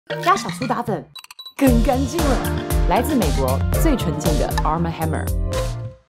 Arm and Hammer x Ella Chen - 1.2.Clean
Ella Chen share how Arm and Hammer Baking Soda helps remove stains and odors easily. Pure, safe and natural. More Power to You.